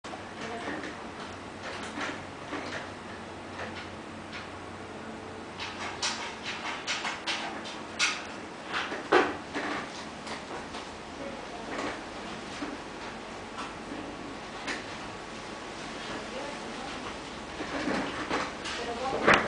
baby snail on my nail!
baby snail crawling on my fingernail